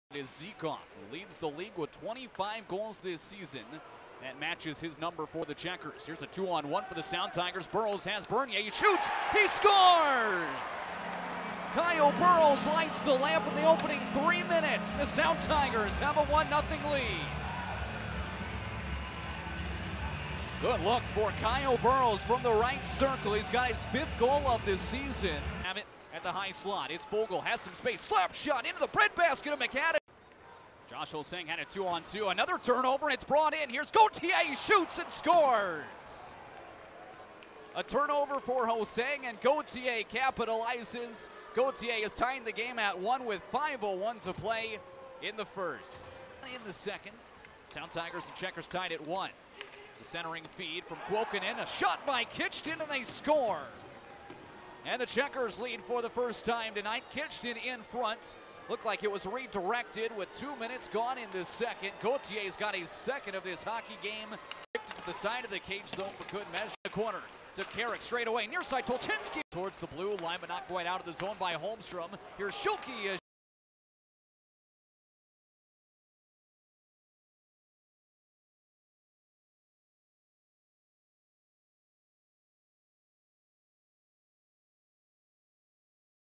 FINAL Checkers 2 - Sound Tigers 1
Sound Tigers come up short against the Checkers by a score of 2 to 1